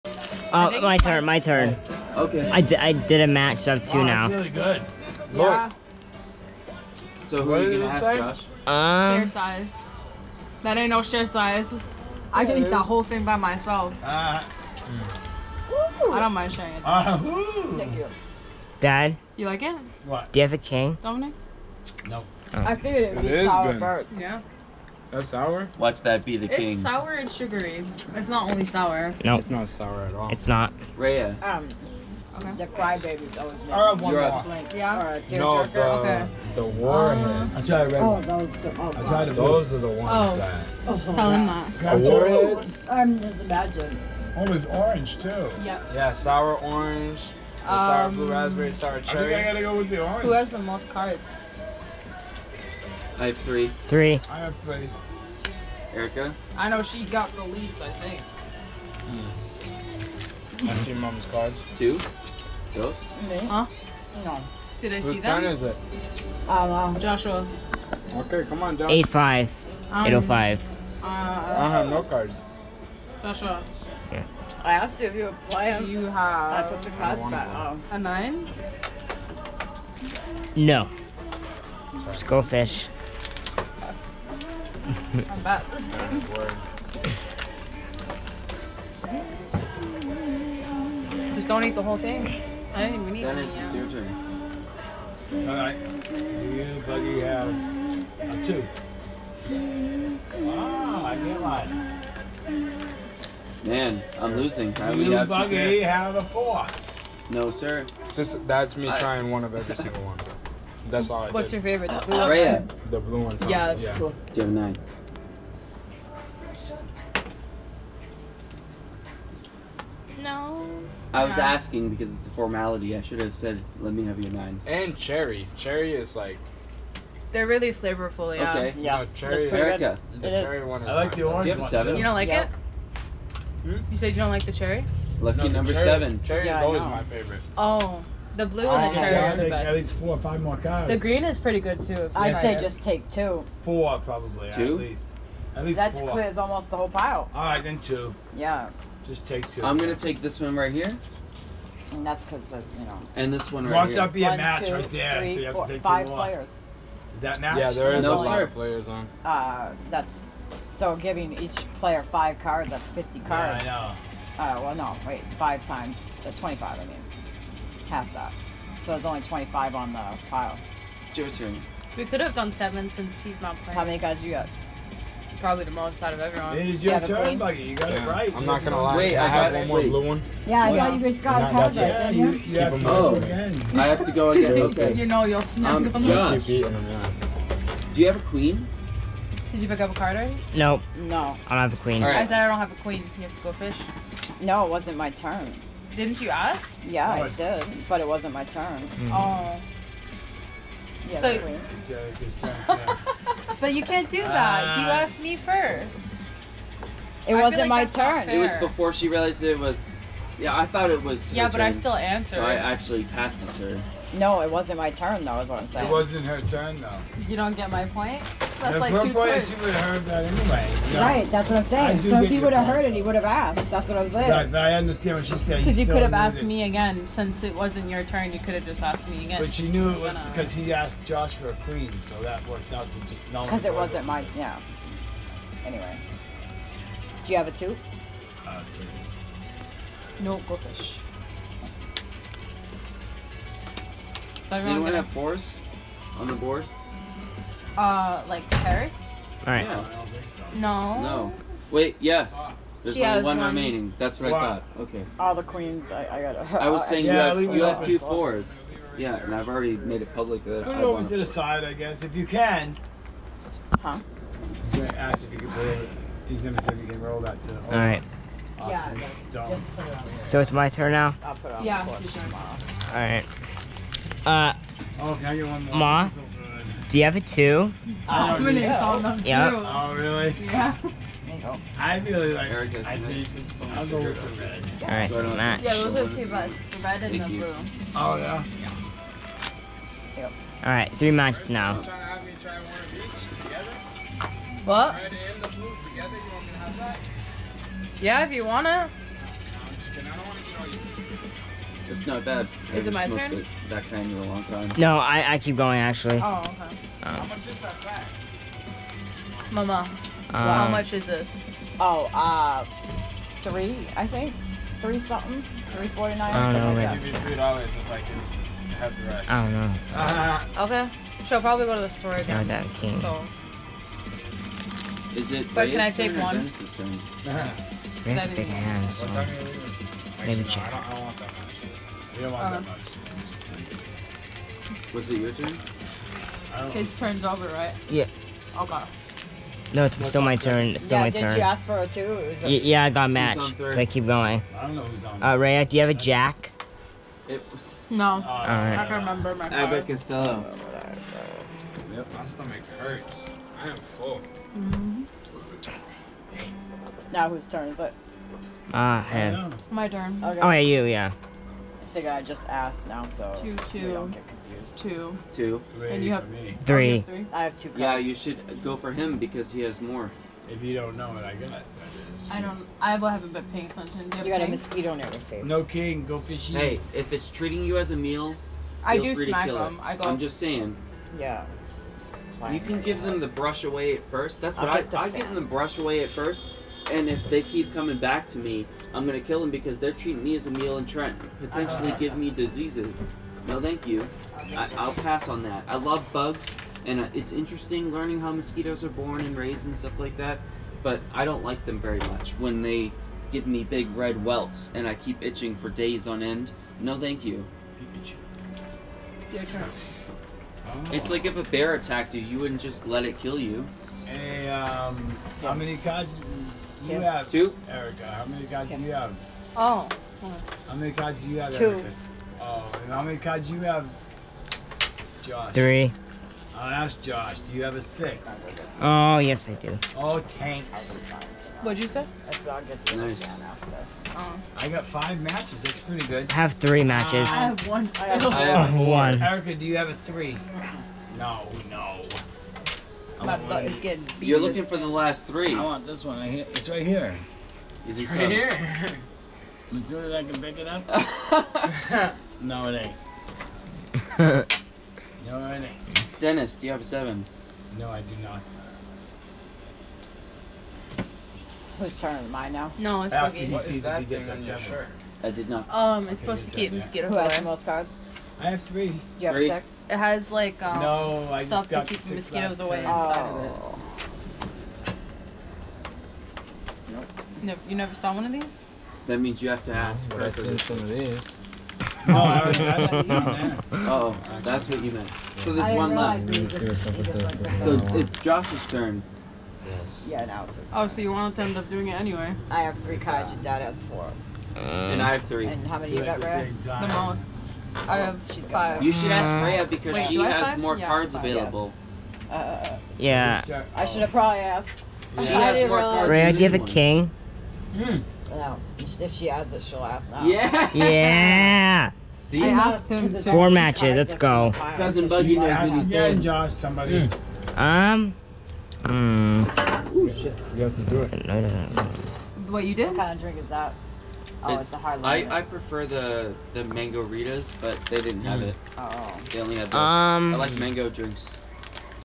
Gopro | Go Fish Footage Dad, Josh & Buggy (Day 57) Continuation
In this video I hang out with dad, Buggy and we play Go Fish! Hope you enjoy!